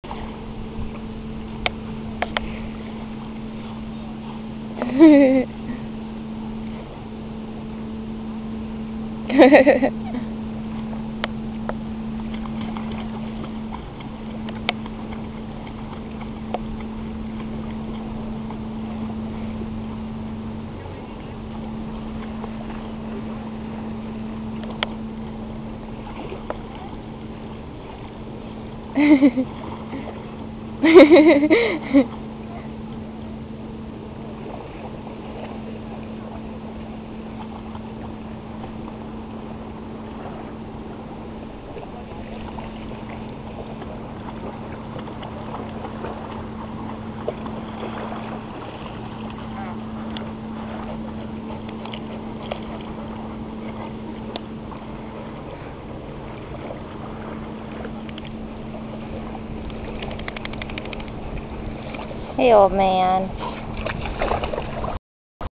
Jay & Ben Beach
benny and jay playing in the water benny flapping aug 2011